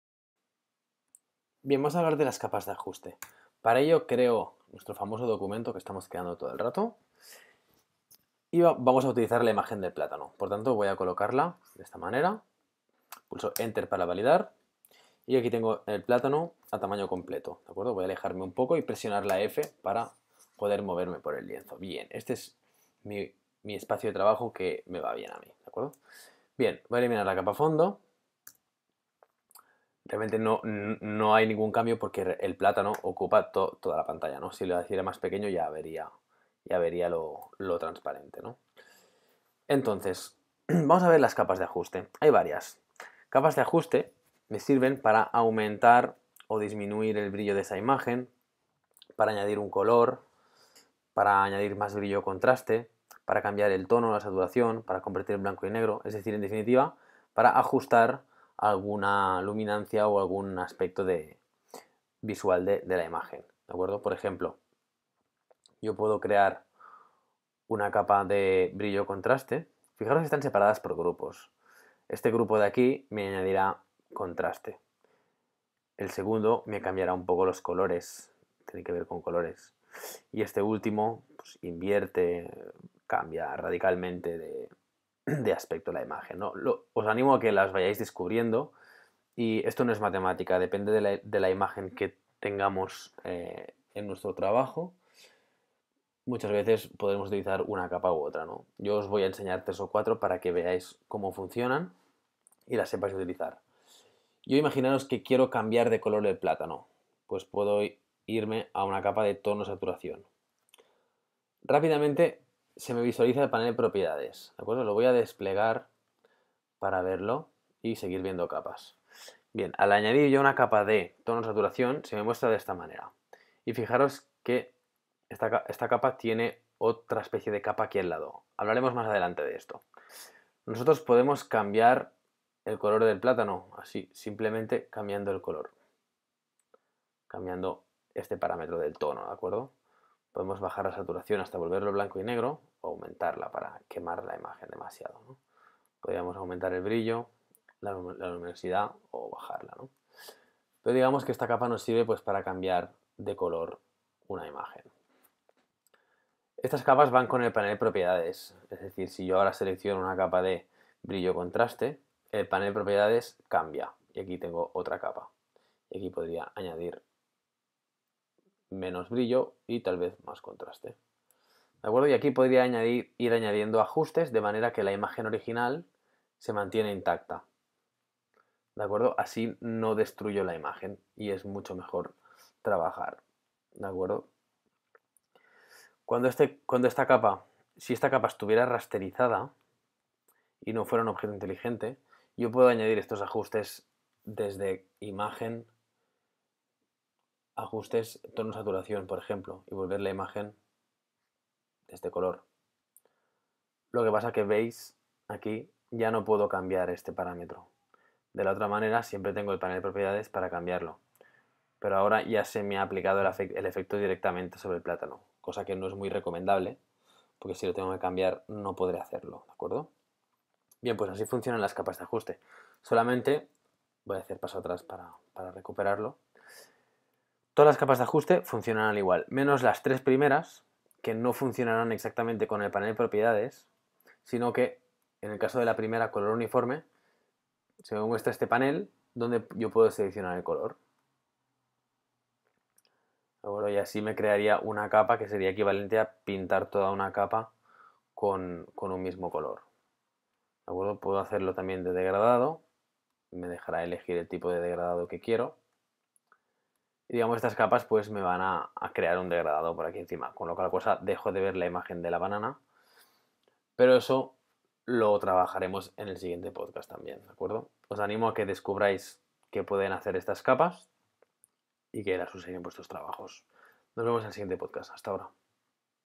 Básicos de Photoshop. Capas de ajuste.
Empieza a descubrir de que va Photoshop y lo que se puede hacer sólo con las opciones de las capas. Domina las opciones que el programa nos ofrece para aplicarlo a tus trabajos.